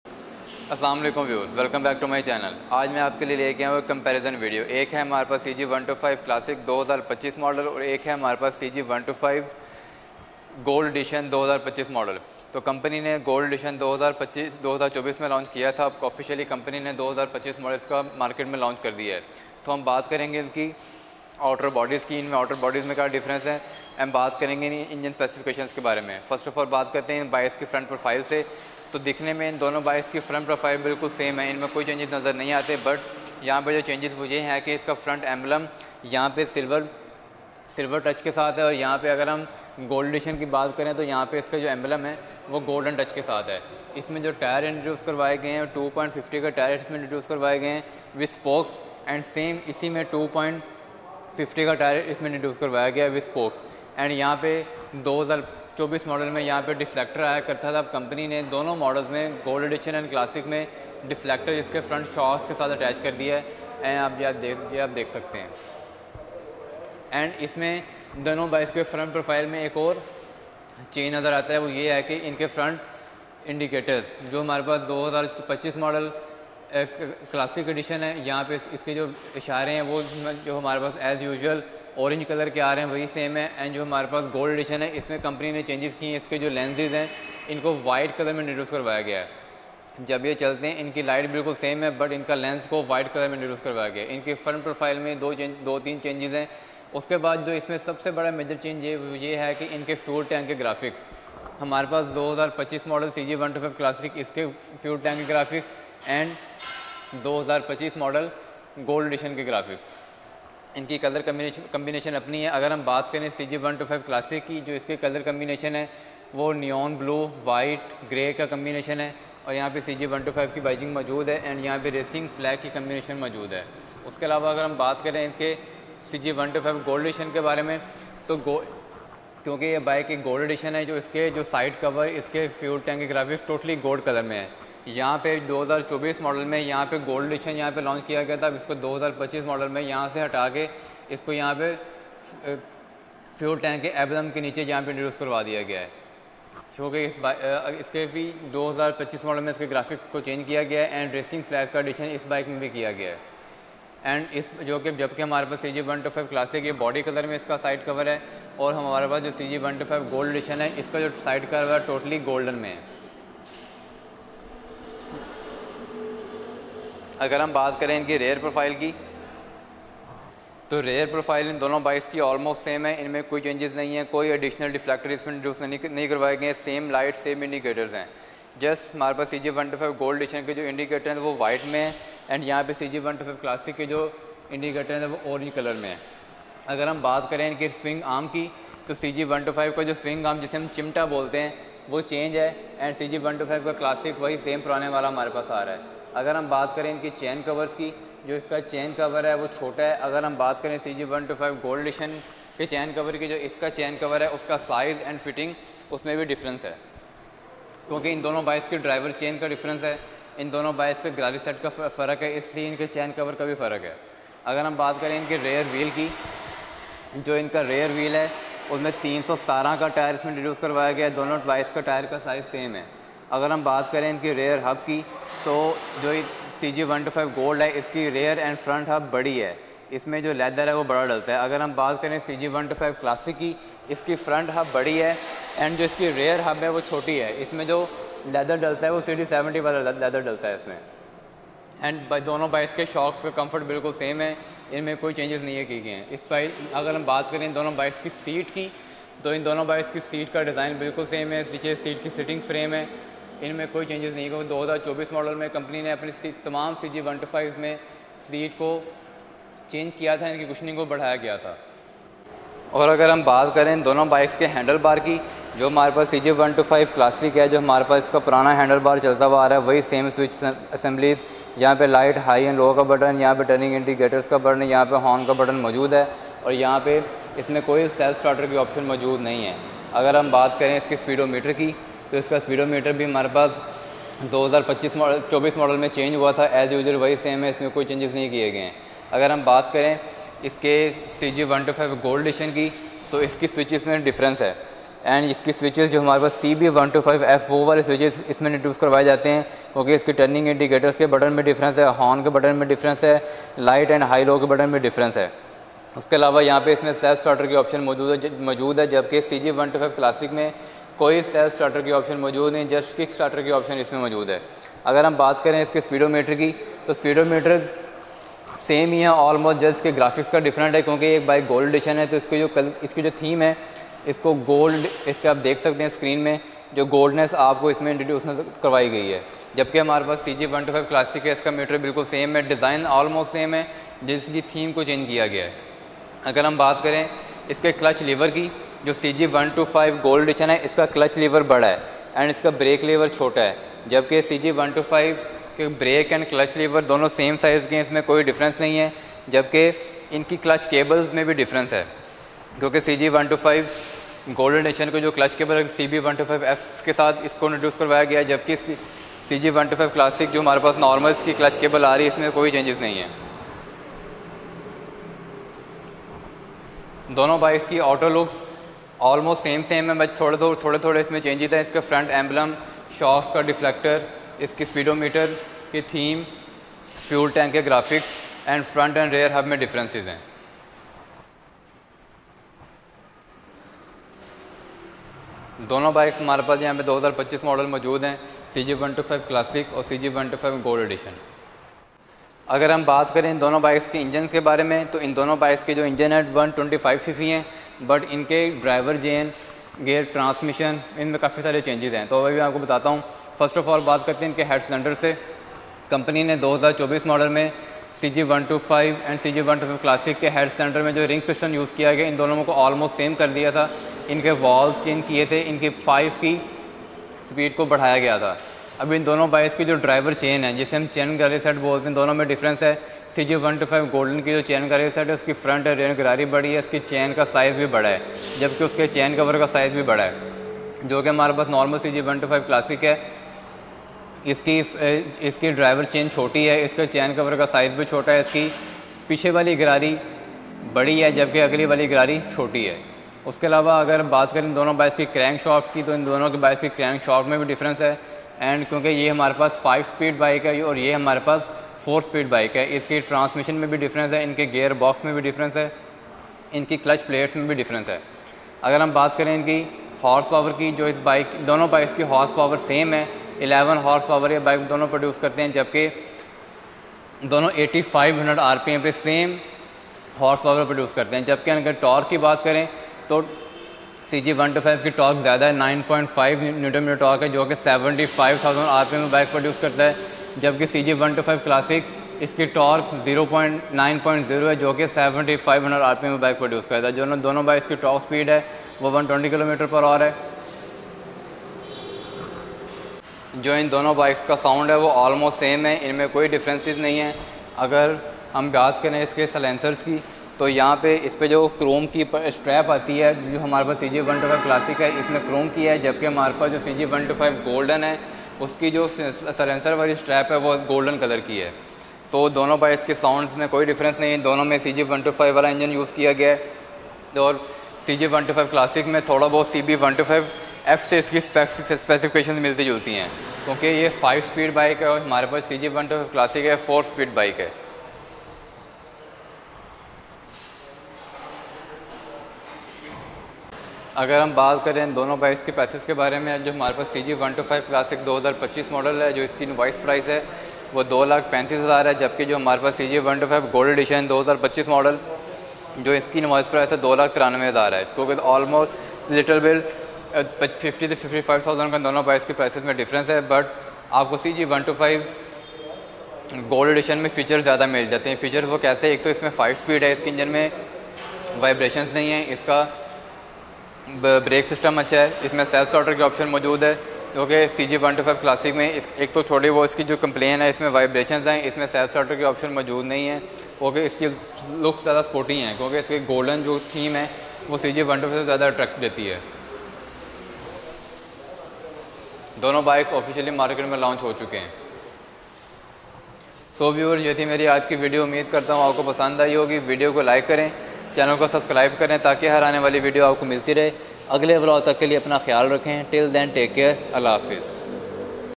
CG125-S GOLD EDITION 2025 MODEL VS CG125 CLASSIC 2025 MODEL | COMPARISON | REVIEW#cg125#bikes
Today we are doing comparison video of 2 bikes 1st is cg125 classic 2025 model and 2nd is cg125-s gold edition both are 2025 models